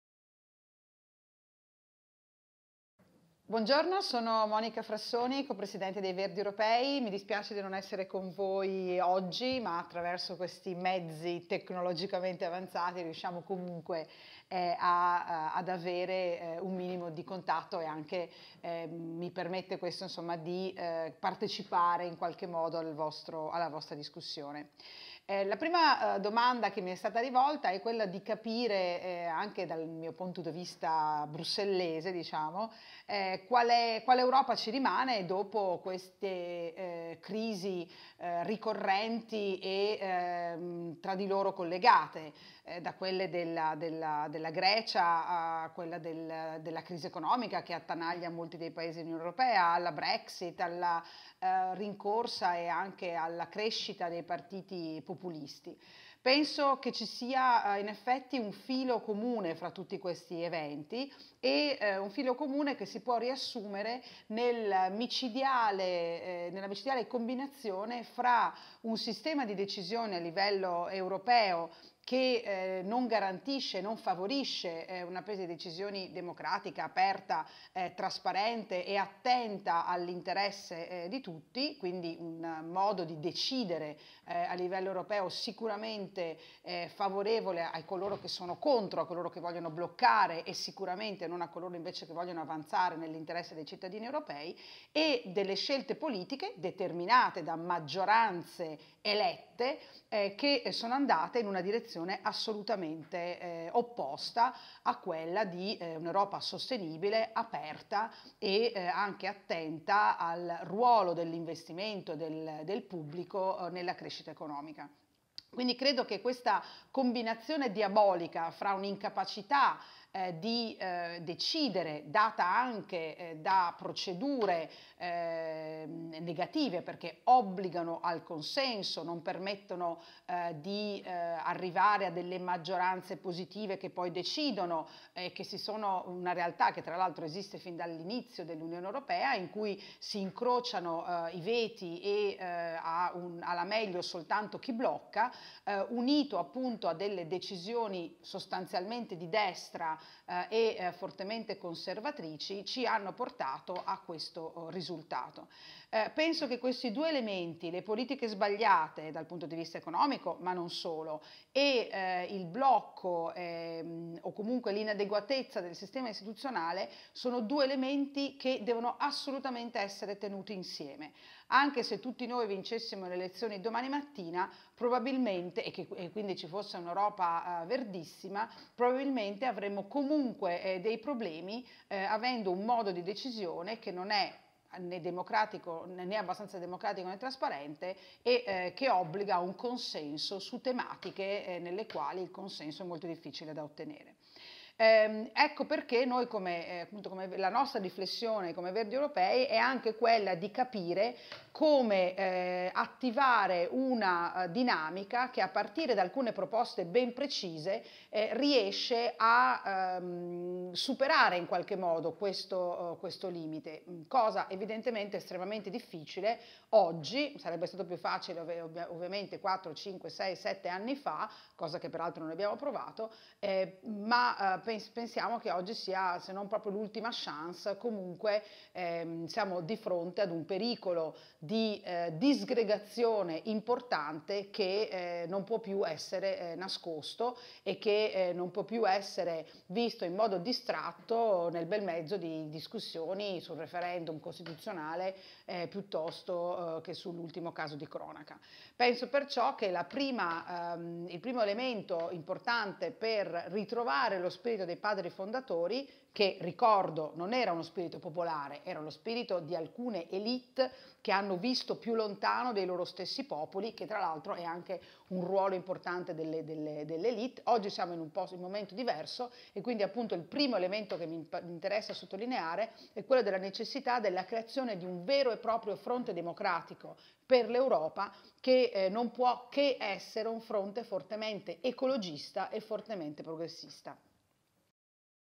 La nostra idea di Europa - Monica Frassoni (Verdi Europei) alla Città Futura in Festa 2016
A fronte dei recenti ed eclatanti fatti internazionali che hanno riguardato lo scorso anno la Grecia, quest'anno la Gran Bretagna (c.d. Brexit) e l'innalzamento dei muri per bloccare i migranti, quale idea di Europa ci rimane? Ovvero si era partiti con una idea dei padri fondatori, che oggi a stento ritroviamo nei fatti. Come riprendere la strada, che sembra smarrita?